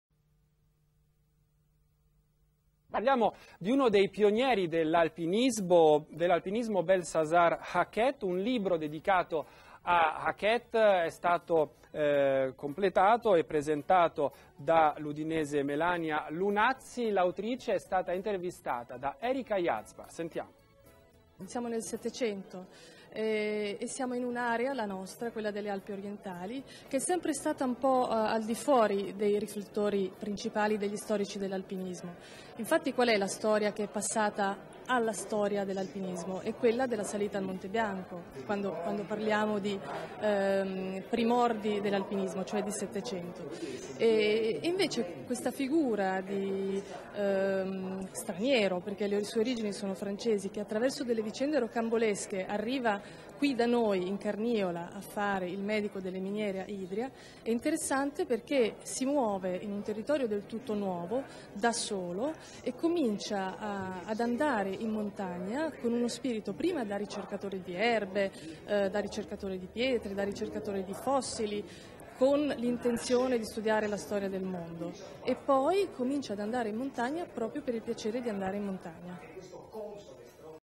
L'esploratore delle Alpi Orientali Belsazar Hacquet, riscoperto nel libro di Melania Lunazzi
Servizio del TGR3 del Friuli Venezia Giulia sul libro di Melania Lunazzi dedicato a primo esploratore delle Alpi Orientali: Belsazar Hacquet. Dal Tricorno alle Dolomiti. Un viaggiatore del Settecento. Autore del primo tentativo di salita al Monte Tricorno, la vetta più alta della Slovenia, Hacquet fu autore di una scoperta ben più importante, molto prima del giovane Dolomieu, che incontrò a Lubiana nel 1784: quella che sarebbe diventata la "dolomia" era nota per le sue caratteristiche di scarsa effervescenza agli acidi già a Belsazar. Per un diverso corso della fortuna le Dolomiti odierne, si chiamano così: avrebbero potuto chiamarsi Hacquetiti.